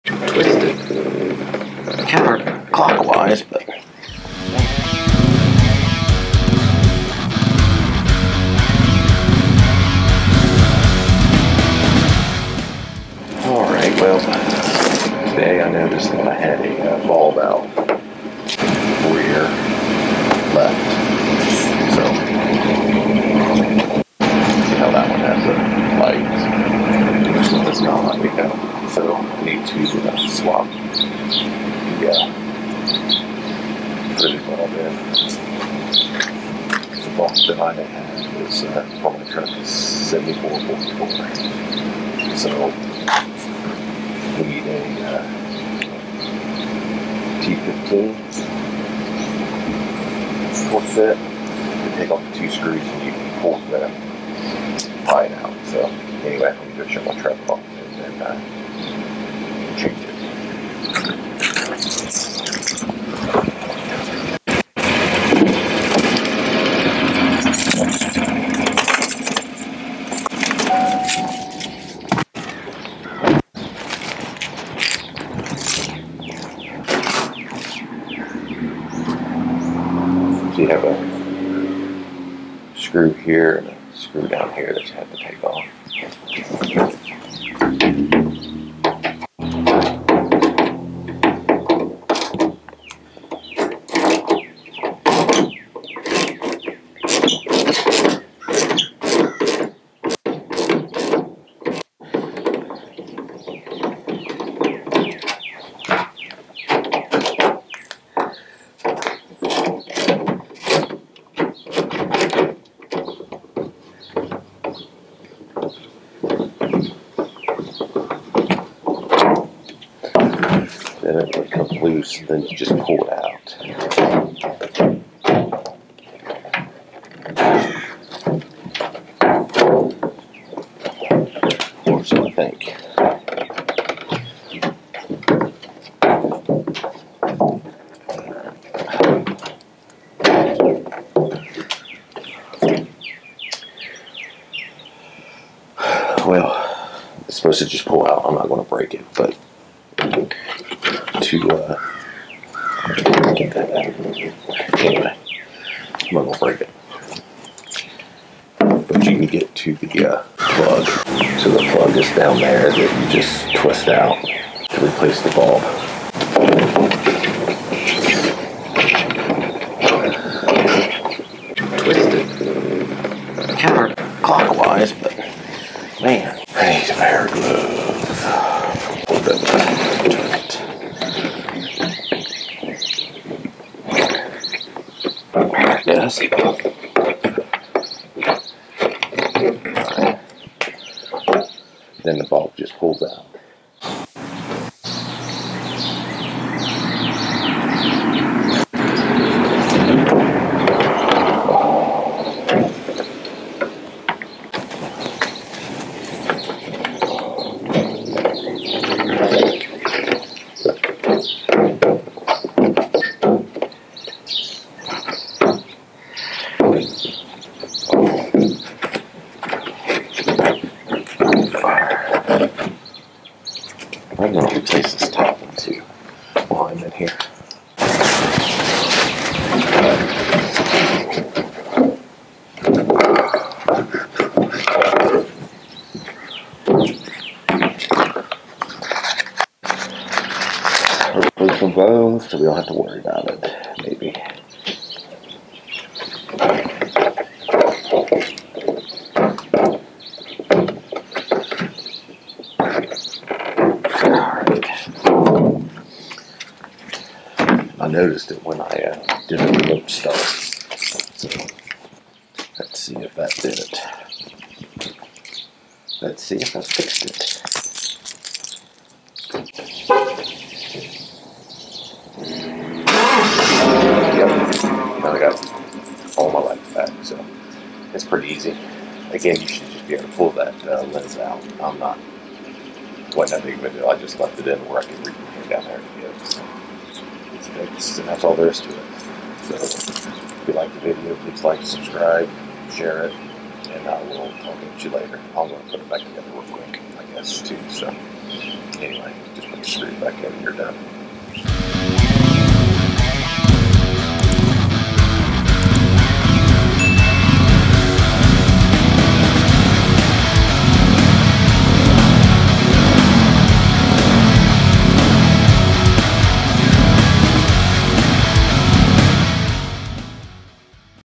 I had to change my taillight bulb on my 2014 Chevy Silverado.
Changing my taillight bulbs on the left side on my Chevy Silverado.

GoPro Hero 8

Mics:
Moukey Universal Video Microphone
Purple Panda Lavalier Lapel Microphone
Rode VideoMicro
Inline Volume Control for 3.5MM Headphones